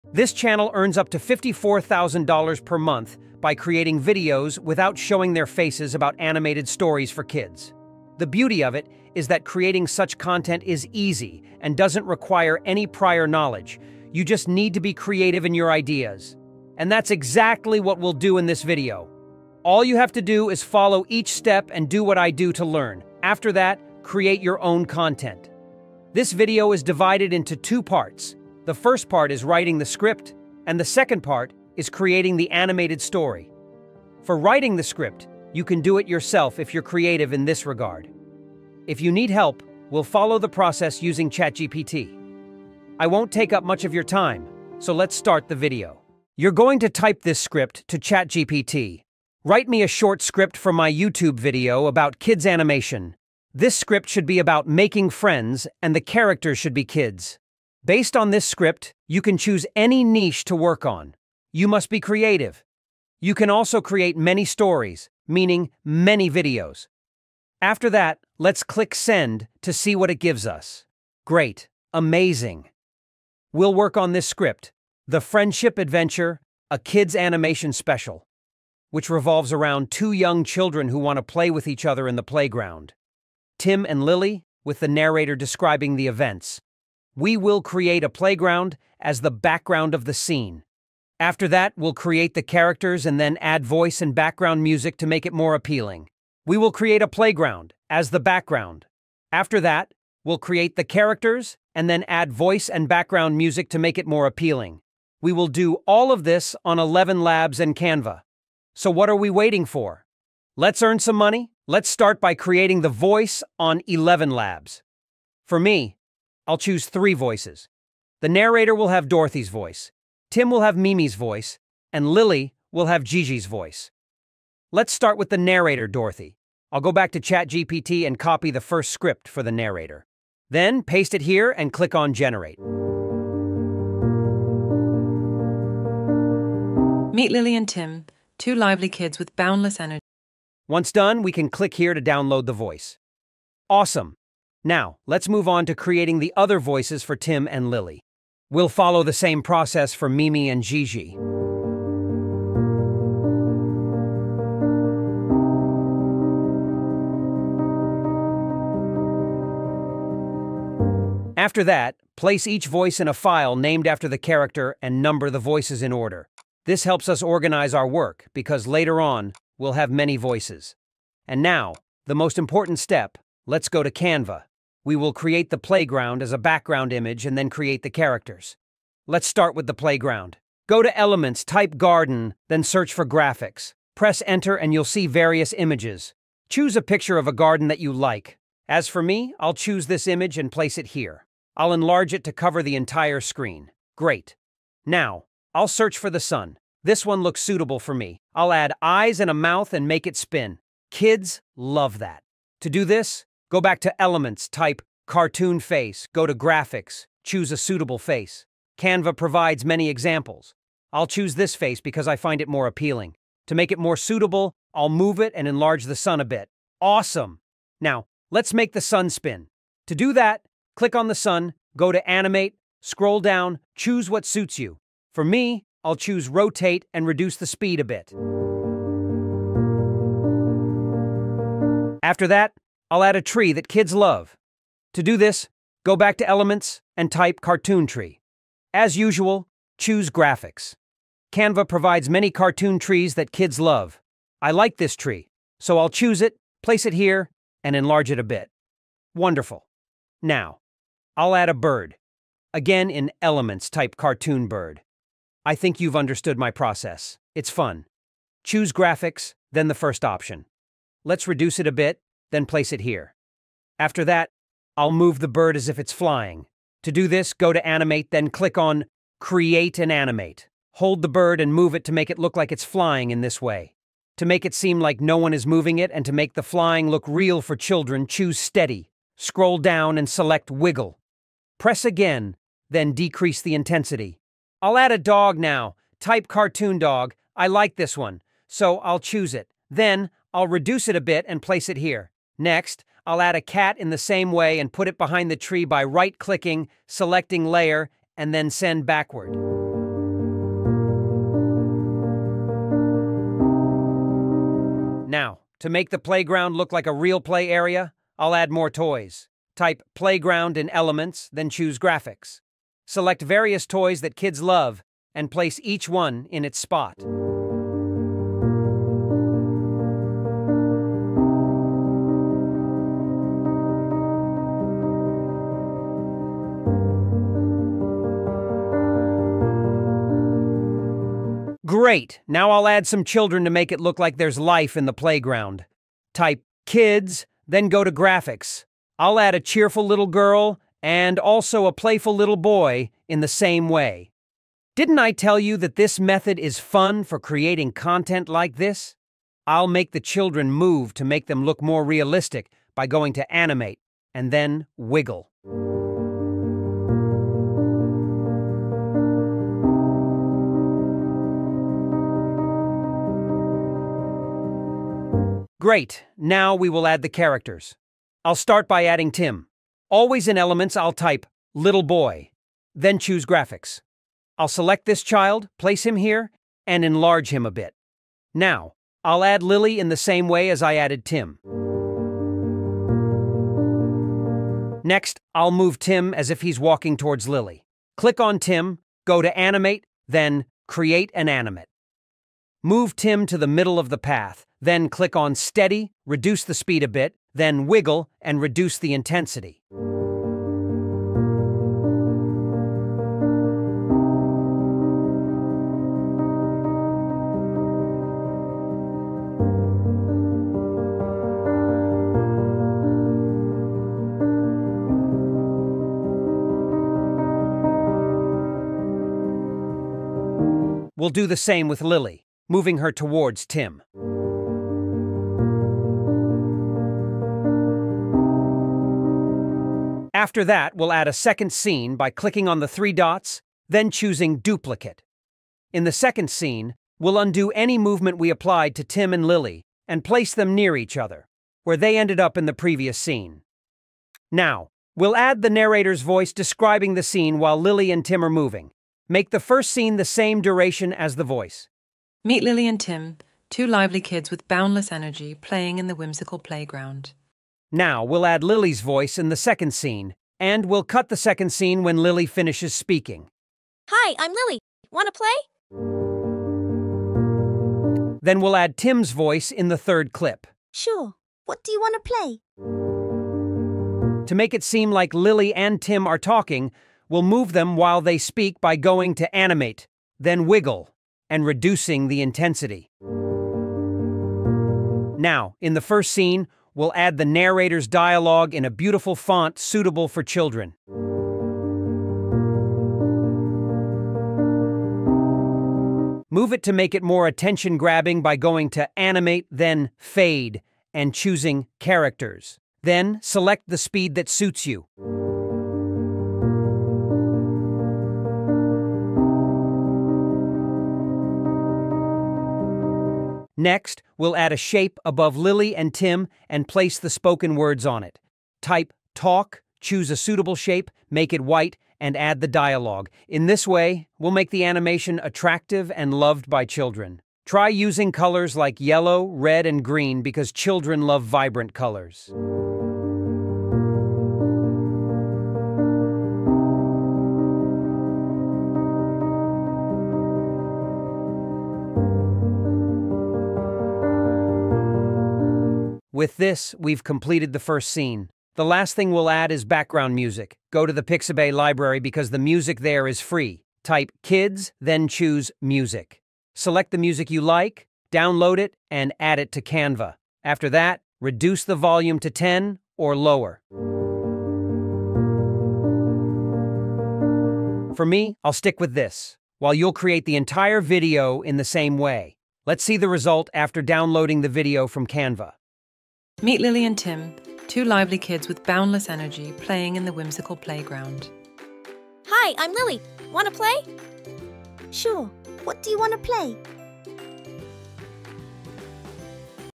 How to Create a FACELESS YouTube Video on KIDS ANIMATION in Minutes Using Canva & AI Tools
How To Make FACELESS KIDS ANIMATION Channel Videos In Minutes Using Ai Tools & Canva
In this video, I'll guide you through creating faceless kids' animation videos using AI tools and Canva for a unique YouTube channel.
Some channels in this niche are making up to $1000 PER DAY.
So, if you're interested in starting a channel like this, I'll show you the steps to get started!